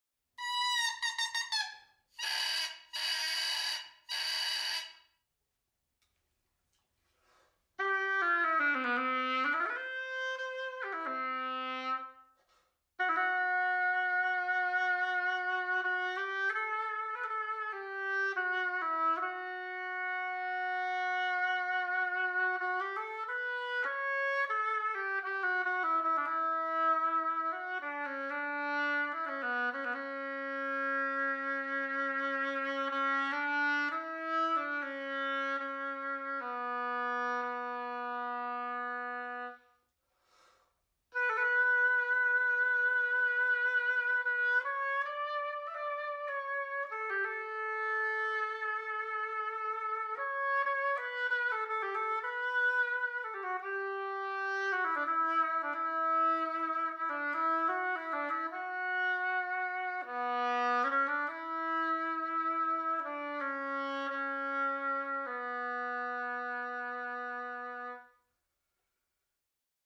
Güner Oboe Cane
This is my very first reed with Güner reeds from Turkey. I have only worked on a few of them so far. I tied two reeds and scraped the one that seemed the least beautiful. And I am amazed at the result! A warmth and velvety feel that I haven't had like this for a long time. After that it was only the 1st reed: maybe it was beginner's luck? 😅

Ceci est ma toute première anche avec les roseaux Güner qui viennent de Turquie. Je n'en ai travaillé que quelques uns pour l'instant. J'ai monté deux anches et gratté celle qui me semblait la moins belle. Et je suis stupéfait du résultat ! Une chaleur et un velouté que je n'avais pas eu comme ça depuis longtemps. Après ce n'est que la 1ère anche : peut-être était-ce la chance du débutant ? 😄

Simple sound recording with my iphone 😉